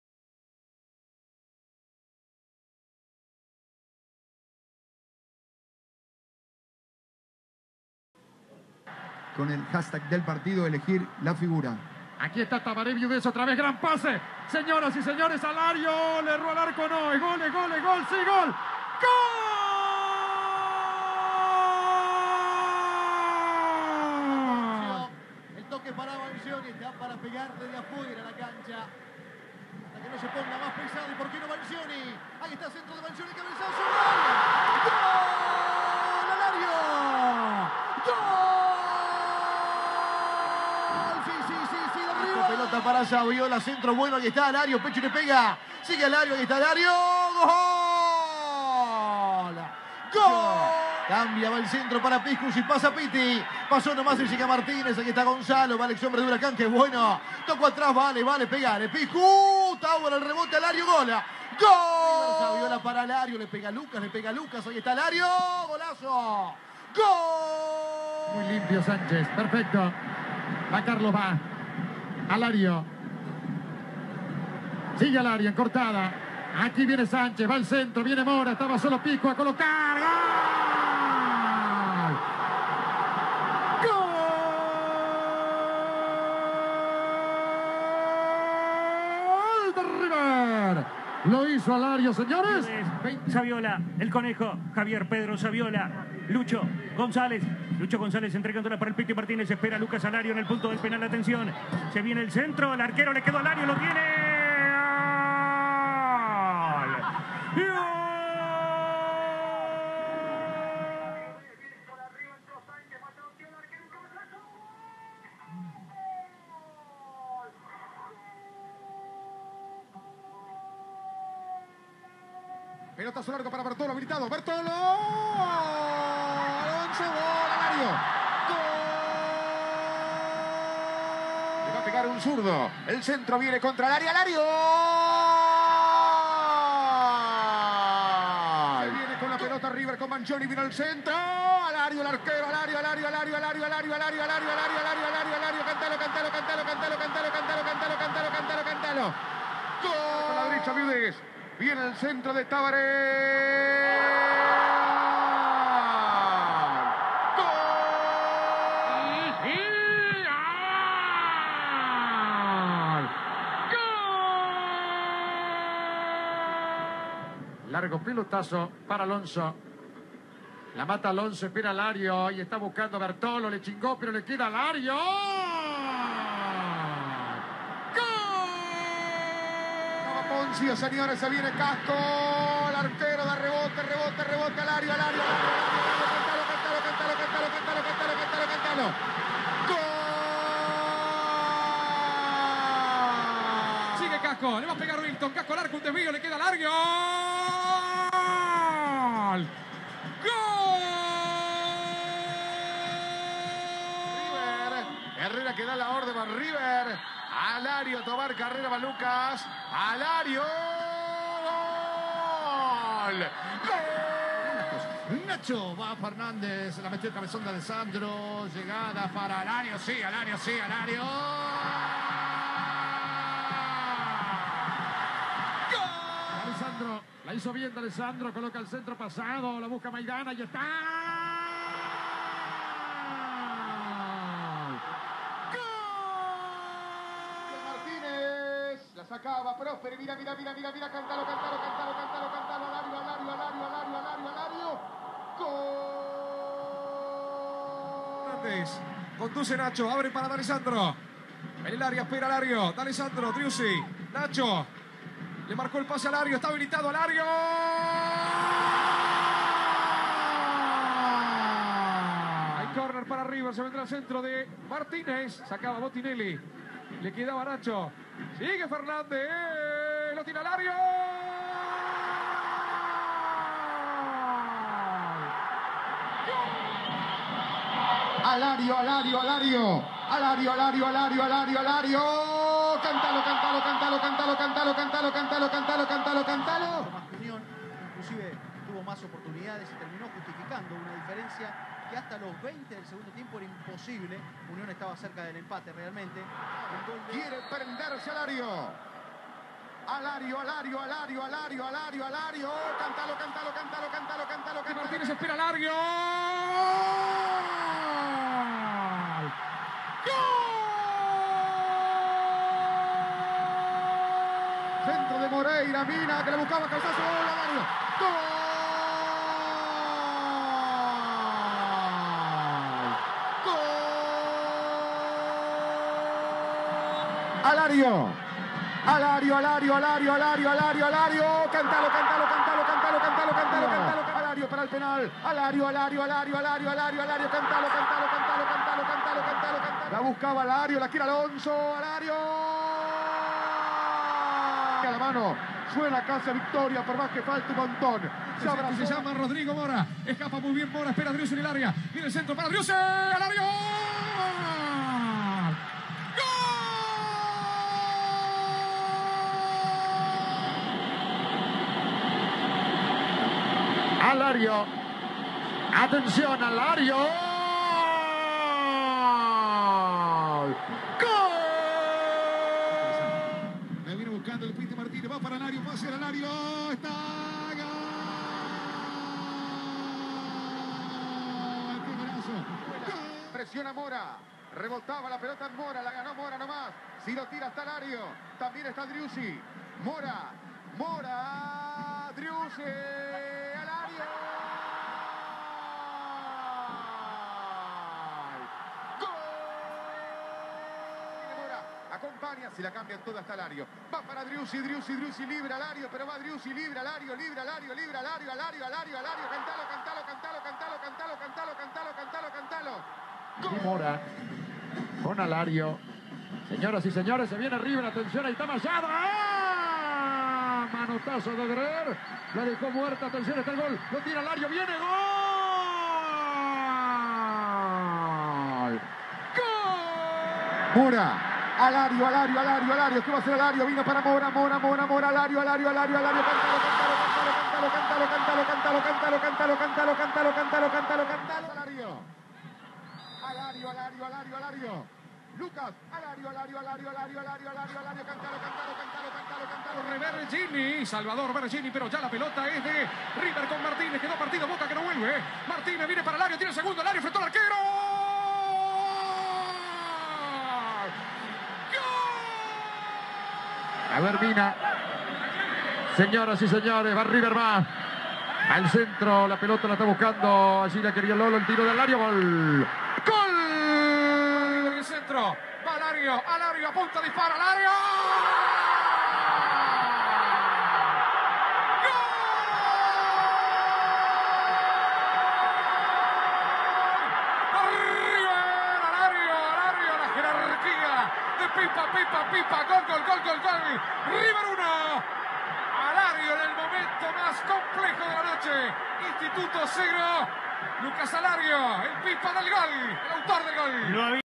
Goles de Alario en River -TODOS- Gracias Pipa!
Todos los goles de Lucas Alario en River (41), quien parece que está cerca del Bayer Leverkusen, aunque todavía mantengo las esperanzas de que se quede. De cualquier manera... ¡Gracias por todo, Pipa!